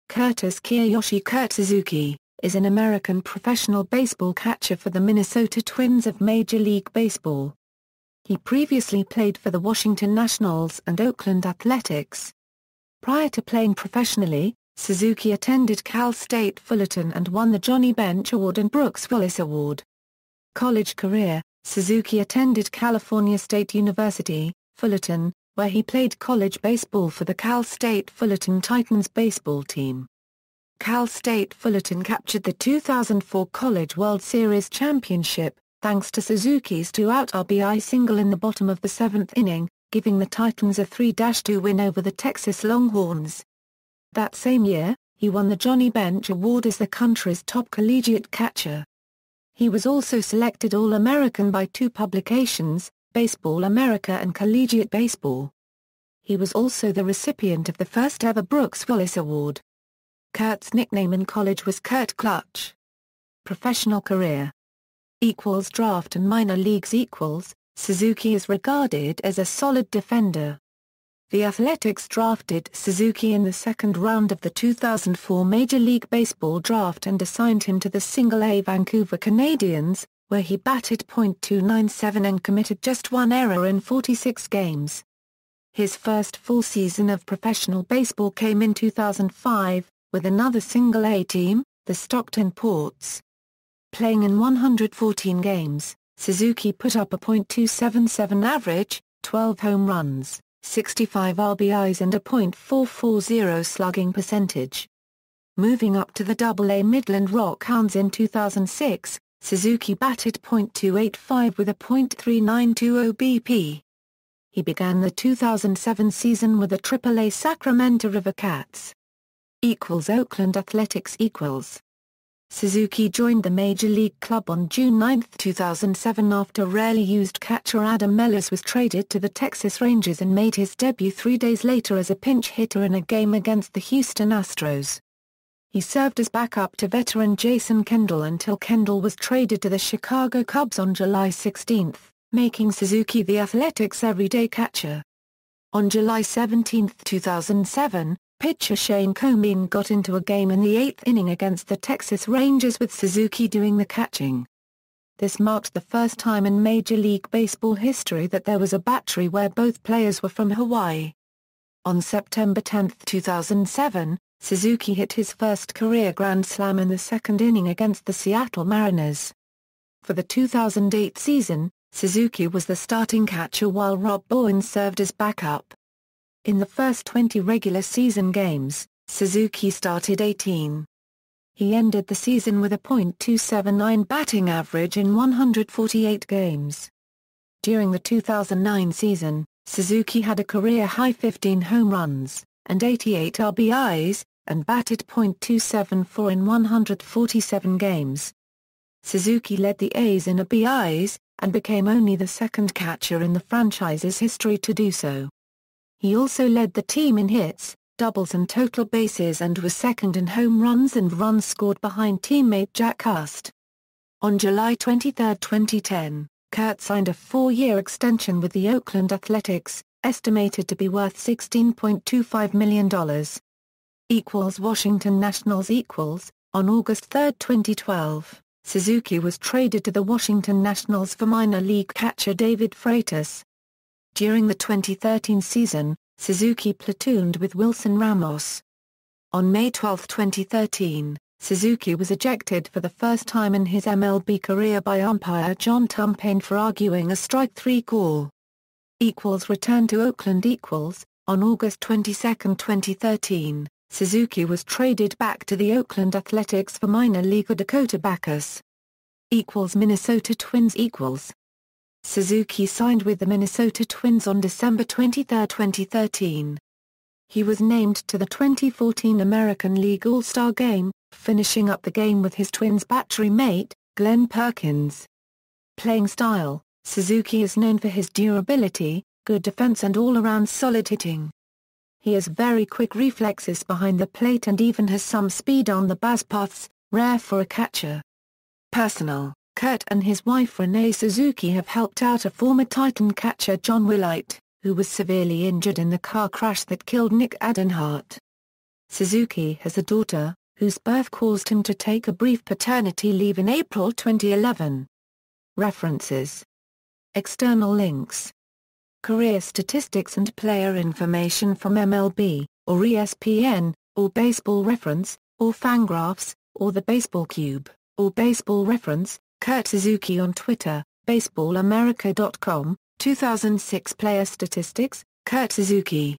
Kurt Suzuki
Kurtis Kiyoshi "Kurt" Suzuki, is an American professional baseball catcher for the Minnesota Twins of Major League Baseball. He previously played for the Washington Nationals and Oakland Athletics. Prior to playing professionally, Suzuki attended Cal State Fullerton and won the Johnny Bench Award and Brooks Wallace Award.